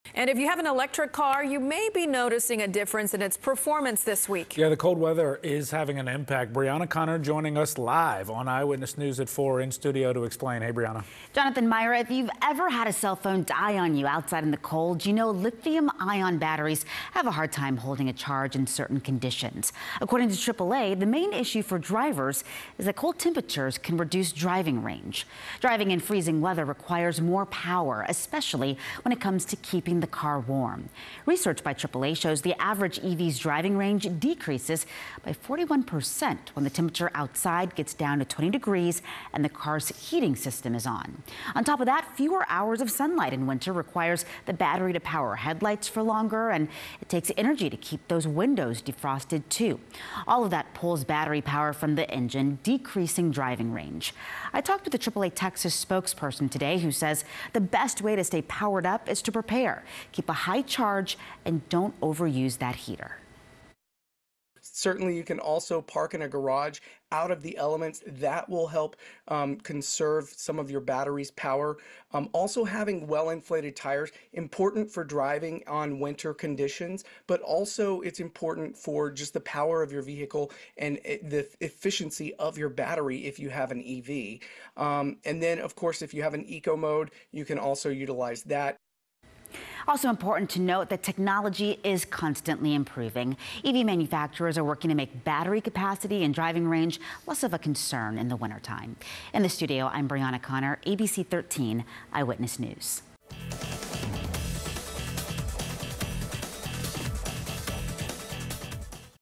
Can cold weather impact electric vehicles?
Decreased driving range in electric vehicles can be a result of cold weather, according to experts.